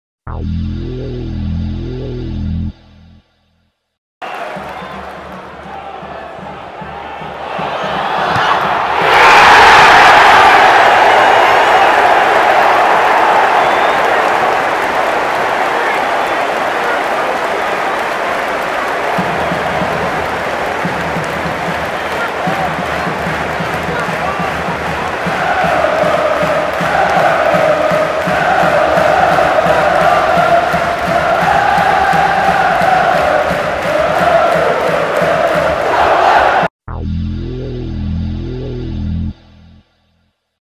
GoPro: Johann Berg Gudmundsson scores against Fulham- Charlton Athletic
Watch Johann Gudmundsson's goal against Fulham from our CAFC Player cameras.